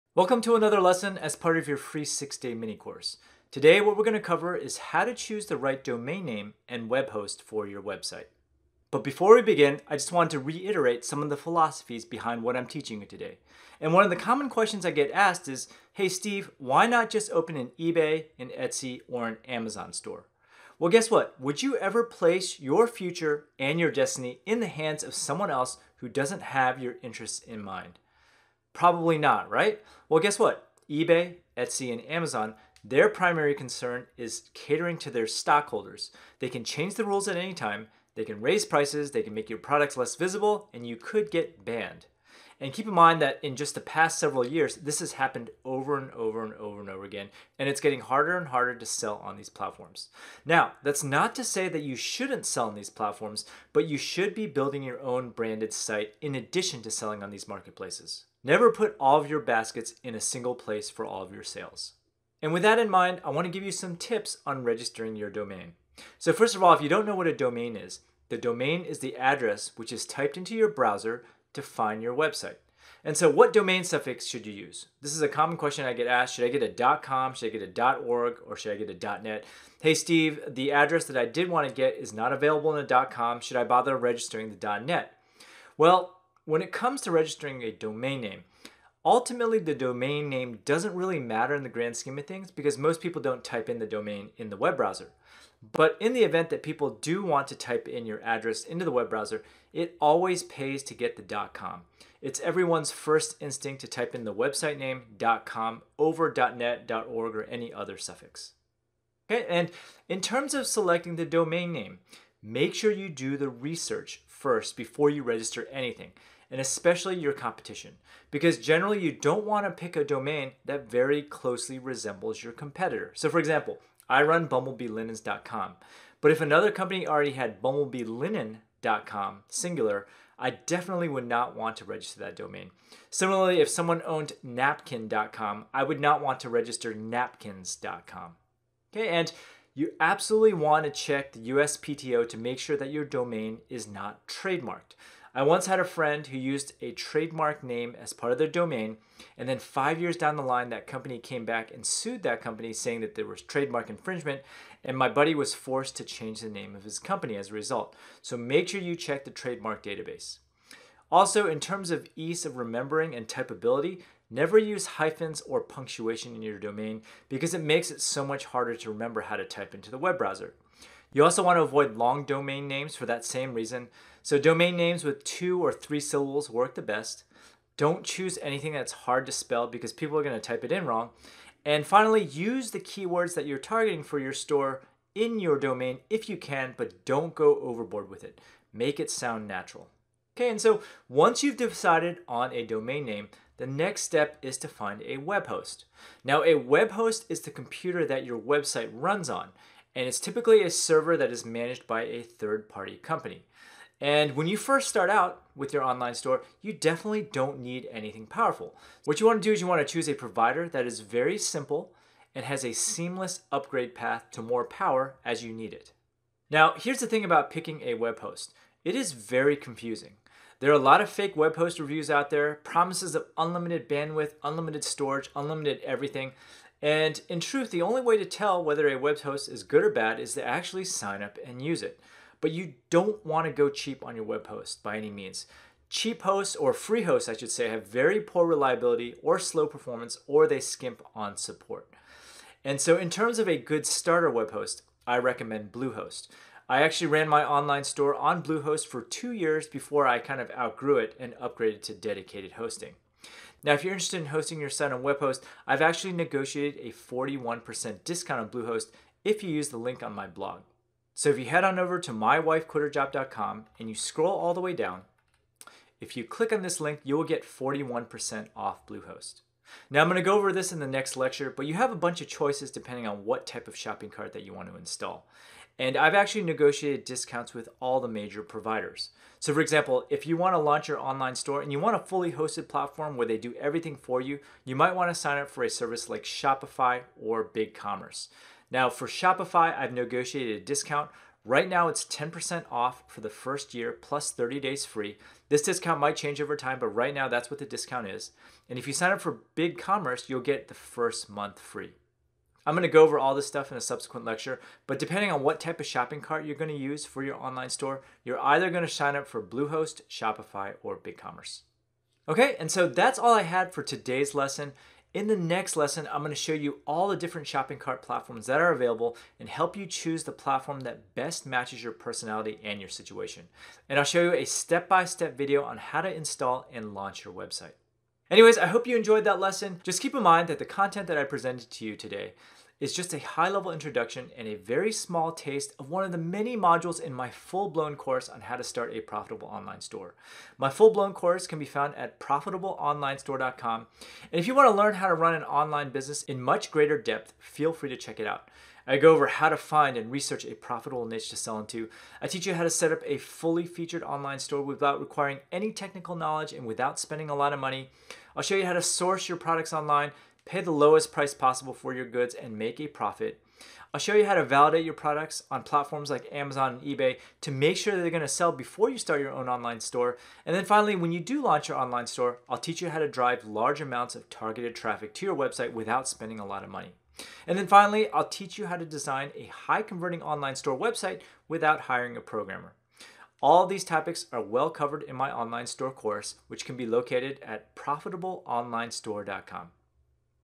How To Choose The Right Domain And Webhost For Your Online Store
How to choose the right domain for your business and how to find the right webhost for your website